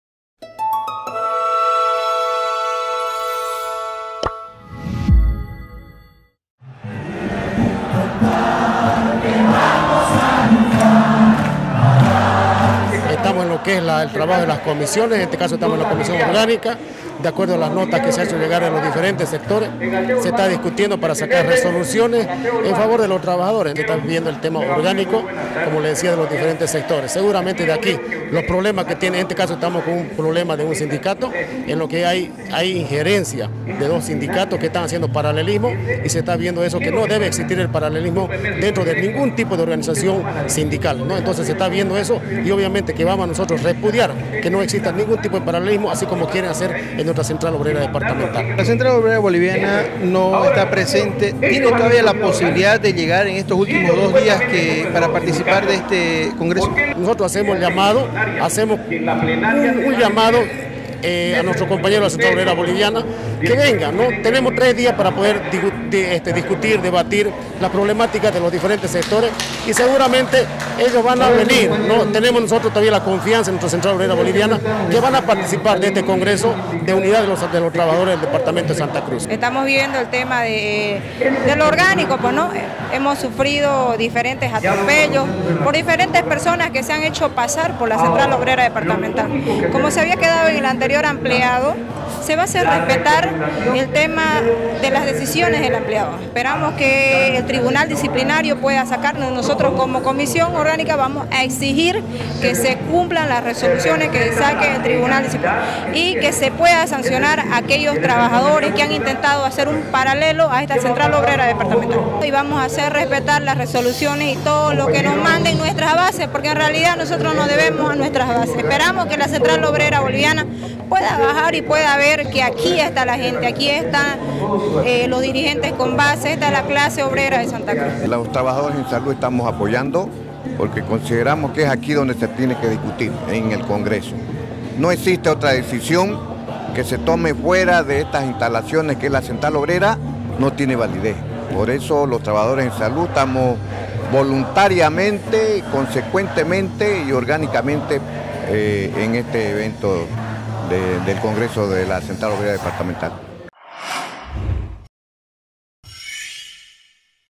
PRIMERA JORNADA DEL CONGRESO DE LA COD CON ÉXITO
Por tiempo y materia se trabaja en las comisiones del XVII congreso de la Central Obrera Departamental en su primera jornada que se lleva adelante con total normalidad, los sectores ponen mucho énfasis en lo orgánico y respeto de los estatutos y un rechazo contundente a los paralelistas en todos los ámbitos.

El parlamento de los trabajadores tiene aún dos días para trabajar y del mismo emanará el nuevo ejecutivo de este ente matriz, cabe hacer notar que han asistido el 95% de los sectores afiliados pese a que autoridades de la cartera del ministerio de trabajo anularon una resolución de comisión transitoria que ya había sido emitida.